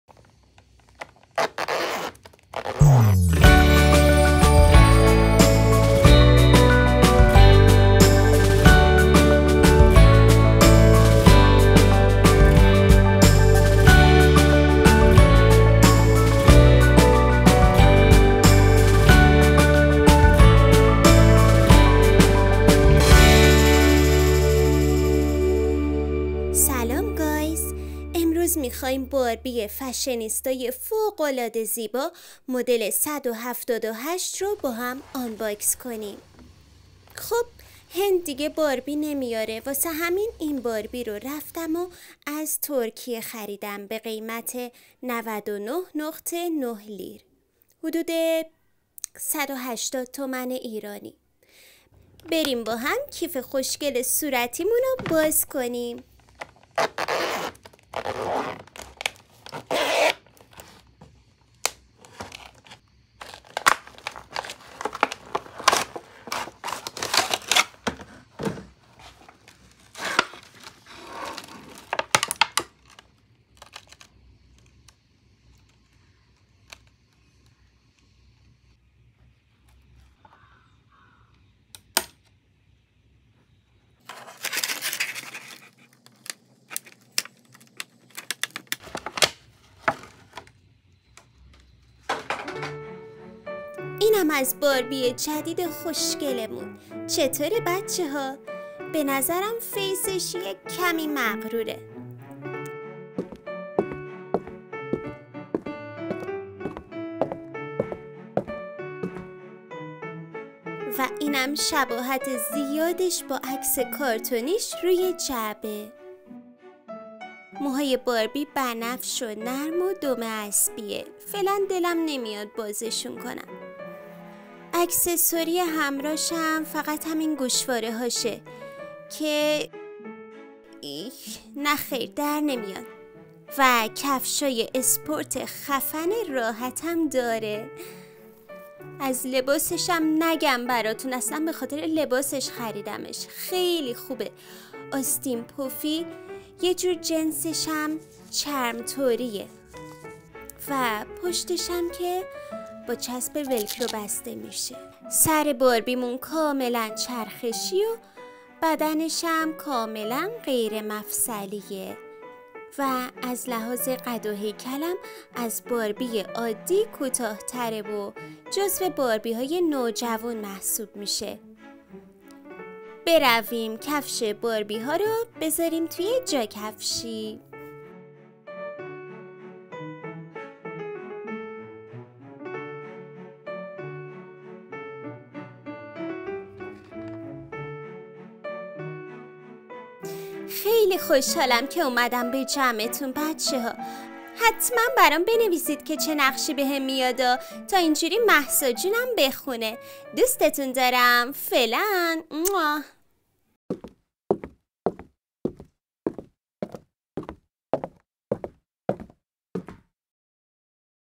آنباکس باربی جذاب| باربی فشنسیتا|داستانهای باربی پاپیون
بریم باهم باربی فشنیستا مدل 178 رو آنباکس کنیم📦✂️ و ببینیم قیمتش چنده💸 و چه اکسسوری هایی داره🤩
unboxing Barbie 178
داستانهای باربی پاپیون